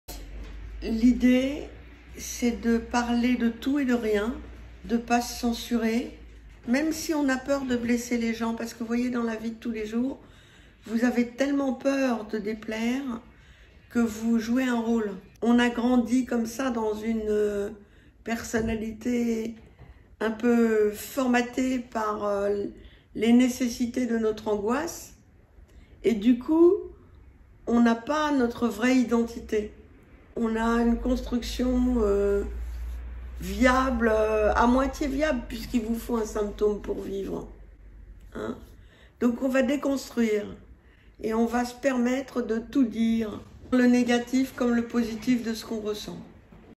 Début d'un groupe: en quoi consiste le travail sur soi en groupe.
C'est parce qu'on se sent vide de soi qu'on a une addiction pathologique. C'est en se trouvant soi que l'addiction pathologique s'en va.